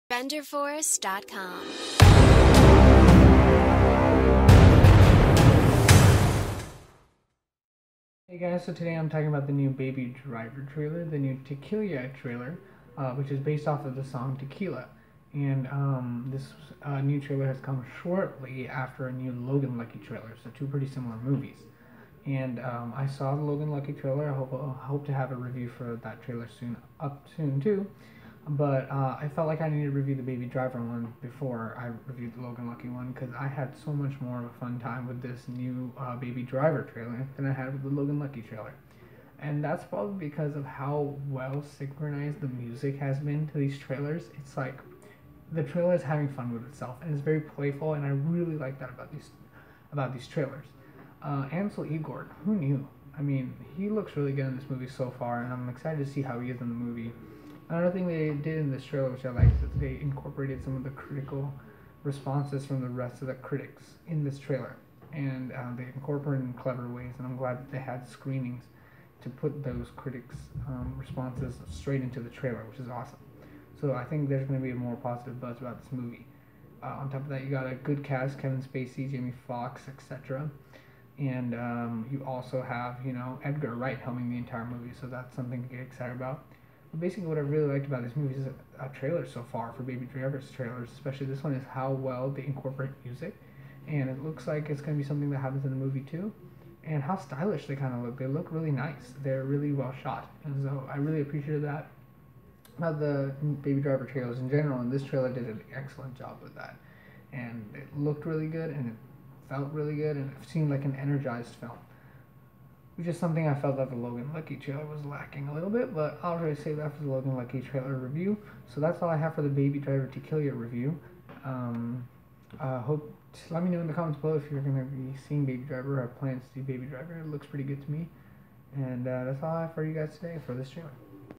BABY DRIVER "TeKillYah" TRAILER: Movie Trailer Review
Rishabh Vashishtha reviews the new BABY DRIVER "TeKillYah" trailer. Enjoy!